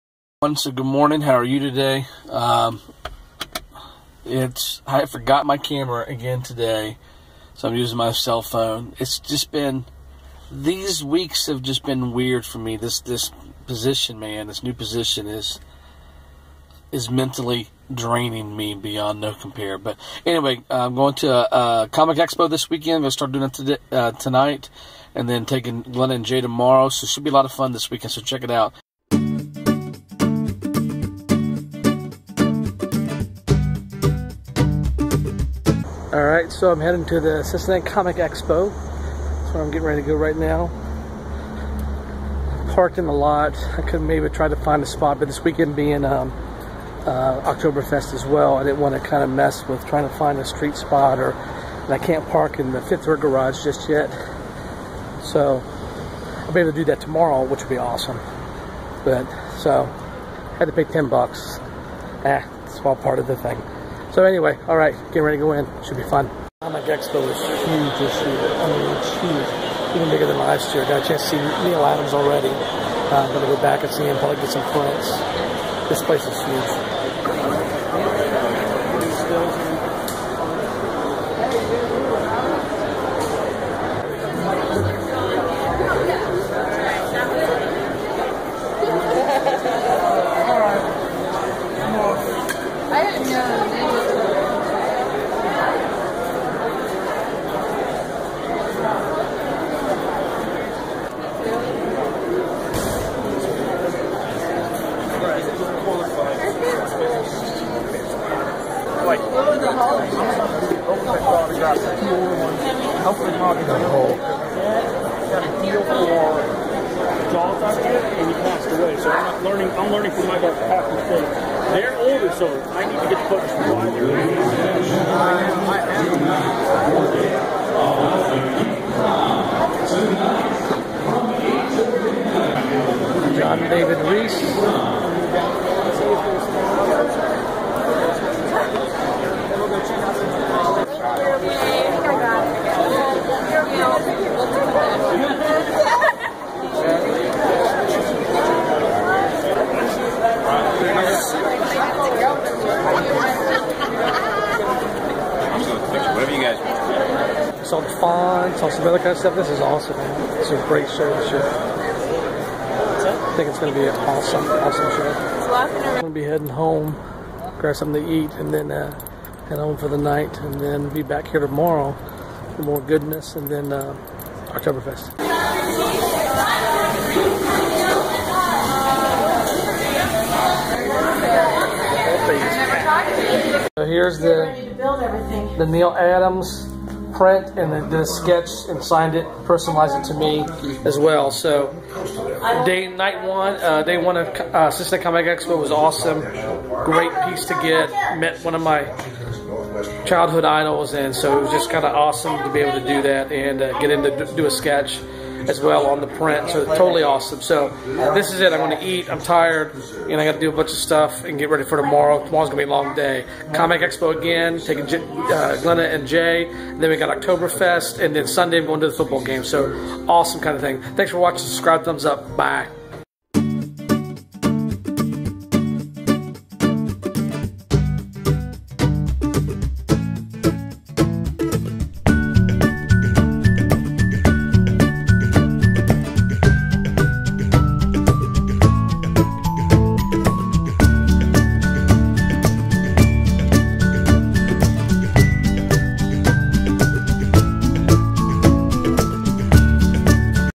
Cincinnati Comic Expo day 1 09-19-2014 day 108
Day one of the comic Expo

I had a blast at day one of the Comic Expo and got to meet Neal Adams!

Featured videos

Heath